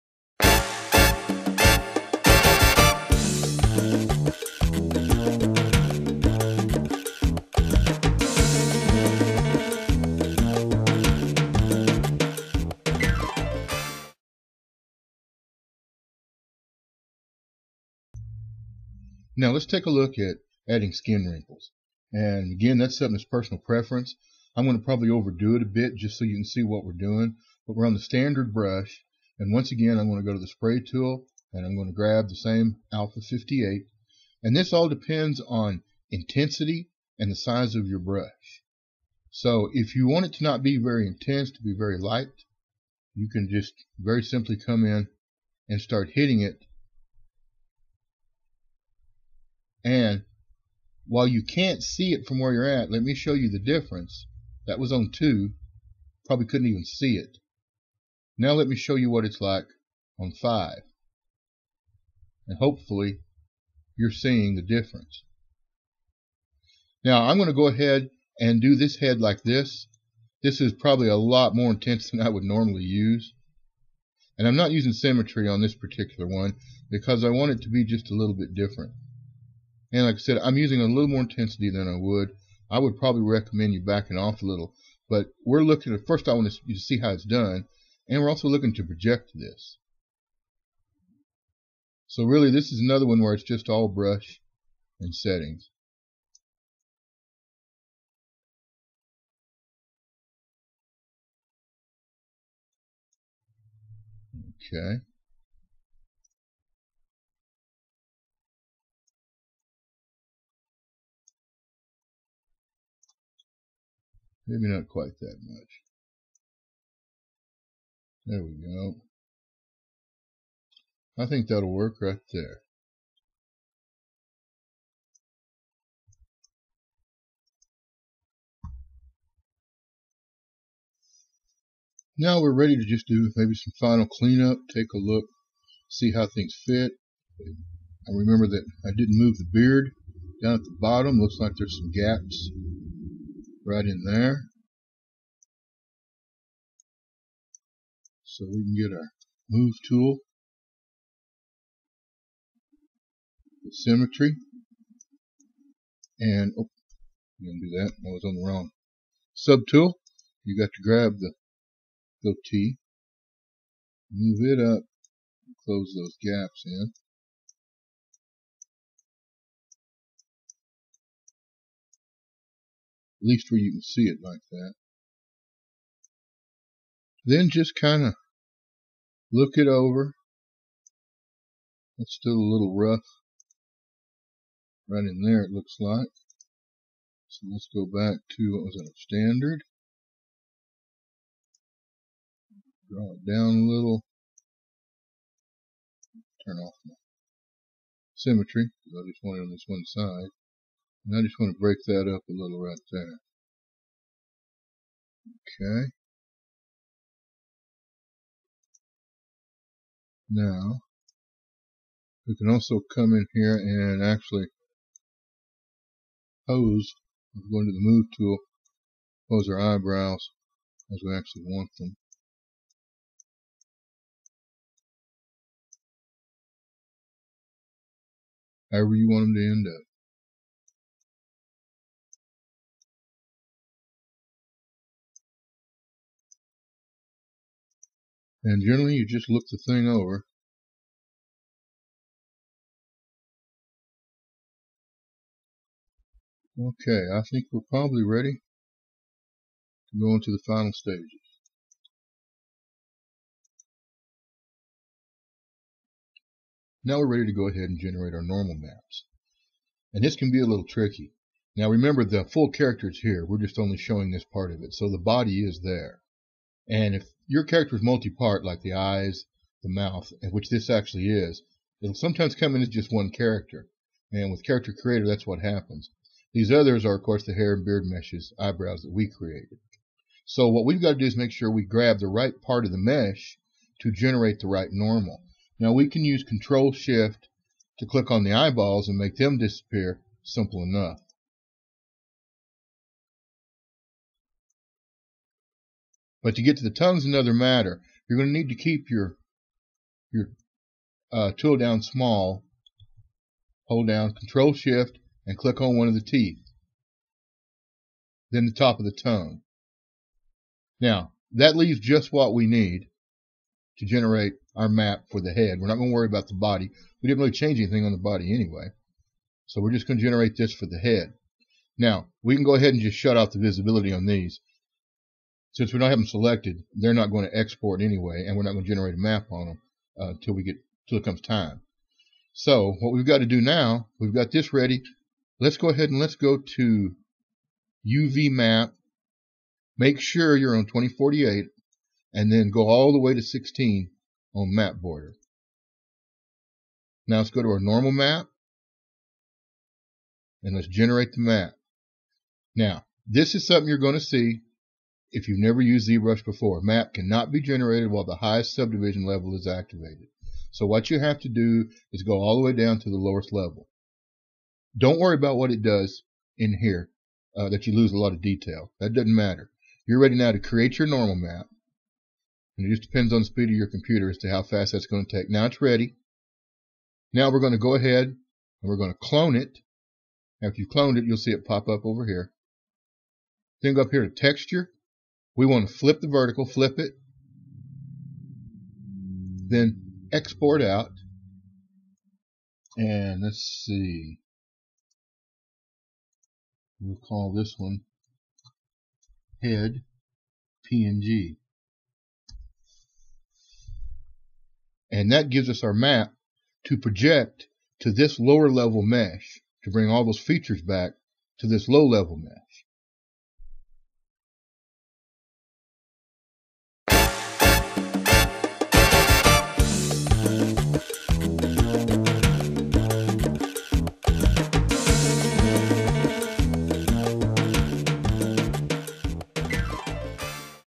Character Creator to zBrush Pipeline - Part 6
In Part 6 we'll create skin wrinkles and one of the most important steps.. generating the normal map to project the high density sculpt features later in the tutorial series.